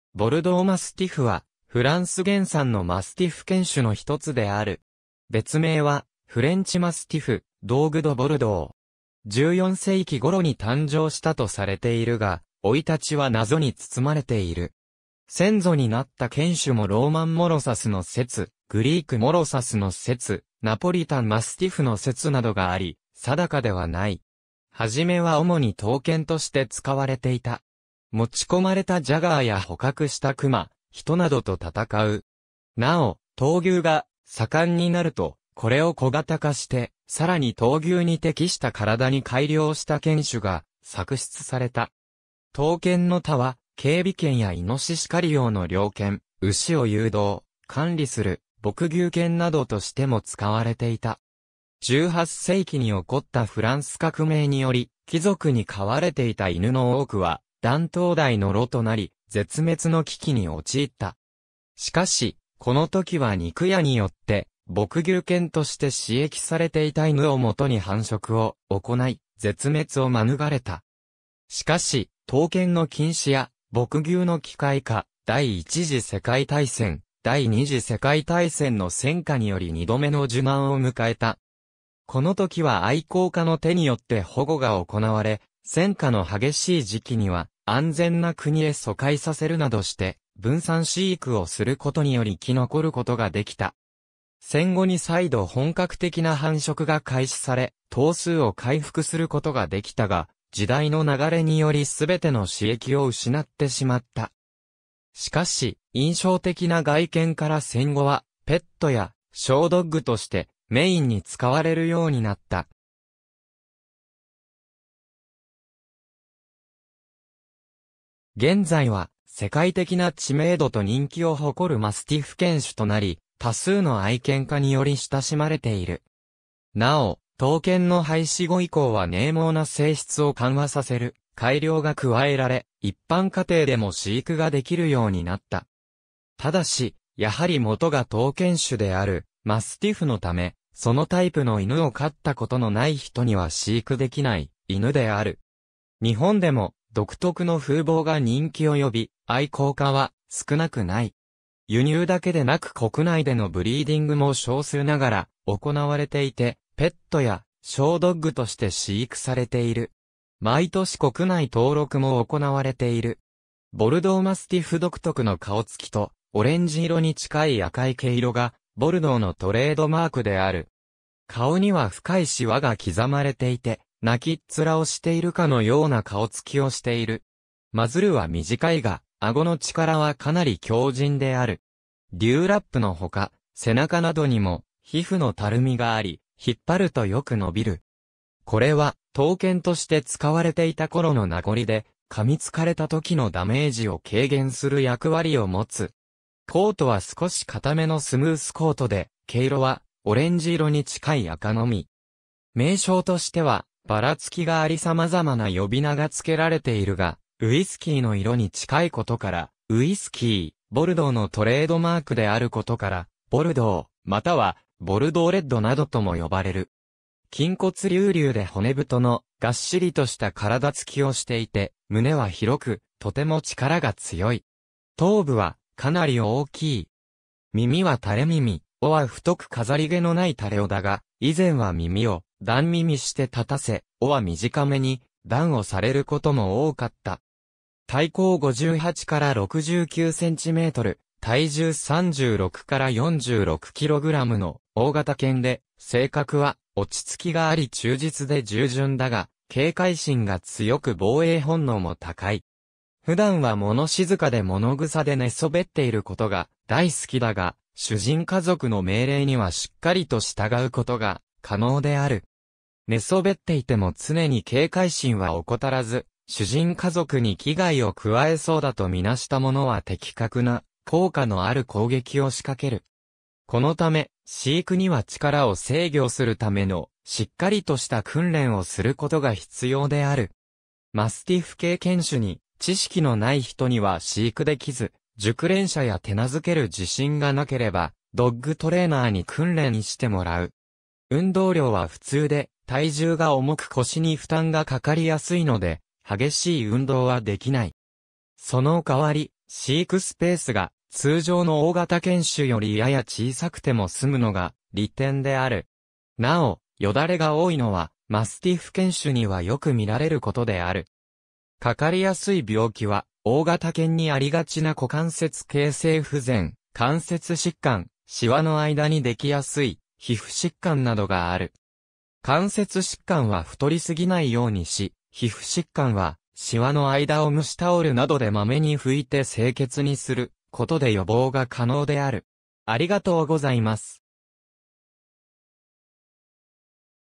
ボルドー・マスティフ
ボルドー・マスティフ（英：Bordeaux Mastiff）は、フランス原産のマスティフ犬種のひとつである。
別名はフレンチ・マスティフㅡ（英：French Mastiff）、ドーグ・ド・ボルドー（英：Dogue de Bordeaux）。
14世紀ごろに誕生したとされているが、生い立ちは謎に包まれている。
先祖になった犬種もローマン・モロサスの説、グリーク・モロサスの説、ナポリタン・マスティフの説などがあり、定かではない。
はじめは主に闘犬として使われていた。
持ち込まれたジャガーや捕獲したクマ、人などと戦う。
尚、闘牛が盛んになると、これを小型化してさらに闘牛に適した体に改良した犬種が作出された。
闘犬の他は、警備犬やイノシシ狩り用の猟犬、牛を誘導・管理する牧牛犬などとしても使われていた。
18世紀に起こったフランス革命により、貴族に飼われていた犬の多くは断頭台の露となり、絶滅の危機に陥った。
しかし、この時は肉屋によって牧牛犬として使役されていた犬を元に繁殖を行い、絶滅を免れた。
しかし、闘犬の禁止や牧牛の機械化、第一次世界大戦・第二次世界大戦の戦禍により2度目の受難を迎えた。
この時は愛好家の手によって保護が行われ、戦火の激しい時期には安全な国へ疎開させるなどして分散飼育をすることにより生き残ることができた。
戦後に再度本格的な繁殖が開始され、頭数を回復することができたが、時代の流れにより全ての使役を失ってしまった。
しかし、印象的な外見から戦後はペットやショードッグとしてメインに使われるようになった。
現在は世界的な知名度と人気を誇るマスティフ犬種となり、多数の愛犬家により親しまれている。
尚、闘犬の廃止後以降は獰猛な性質を緩和させる改良が加えられ、一般家庭でも飼育ができるようになった。
ただし、やはり元が闘犬種であるマスティフのため、そのタイプの犬を飼ったことのない人には飼育できない犬である。
日本でも独特の風貌が人気を呼び、愛好家は少なくない。
輸入だけでなく国内でのブリーディングも少数ながら行われていて、ペットやショードッグとして飼育されている。
毎年国内登録も行われている。
ボルドー・マスティフ 独特の顔つきと、オレンジ色に近い赤い毛色がボルドーのトレードマークである。
顔には深いしわが刻まれていて、泣きっ面をしているかのような顔つきをしている。
マズルは短いが、あごの力はかなり強靭である。
デューラップ（のど下のたるみ）の他、背中などにも皮膚のたるみがあり、引っ張るとよく伸びる。
これは闘犬として使われていたころの名残で、噛み付かれたときのダメージを軽減する役割を持つ。
コートは少し硬めのスムースコートで、毛色はオレンジ色に近い赤のみ。
名称としてはばらつきがありさまざまな呼び名がつけられているが、ウイスキーの色に近いことから「ウイスキー」、ボルドーのトレードマークであることから「ボルドー」または「ボルドー・レッド」などとも呼ばれる。
筋骨隆々で骨太のがっしりとした体つきをしていて、胸は広く、とても力が強い。
頭部はかなり大きい。
耳は垂れ耳、尾は太く飾り毛のない垂れ尾だが、以前は耳を断耳して立たせ、尾は短めに断尾されることも多かった。
体高58〜69cm、体重36〜46kgの大型犬で、性格は落ち着きがあり忠実で従順だが、警戒心が強く防衛本能も高い。
普段は物静かでものぐさで寝そべっていることが大好きだが、主人家族の命令にはしっかりと従うことが可能である。
寝そべっていても常に警戒心は怠らず、主人家族に危害を加えそうだと見なしたものは的確な効果のある攻撃を仕掛ける。
このため、飼育には力を制御するためのしっかりとした訓練をすることが必要である。
マスティフ系犬種に知識のない人には飼育できず、熟練者や手懐ける自信がなければドッグトレーナーに訓練してもらう。
運動量は普通で、体重が重く腰に負担がかかりやすいので、激しい運動はできない。
その代わり、飼育スペースが通常の大型犬種よりやや小さくても済むのが利点である。
尚、よだれが多いのはマスティフ犬種にはよく見られることである。
かかりやすい病気は大型犬にありがちな股関節形成不全、関節疾患、しわの間にできやすい皮膚疾患などがある。
関節疾患は太り過ぎないようにし、皮膚疾患はしわの間を蒸しタオルなどでまめに拭いて清潔にすることで予防が可能である。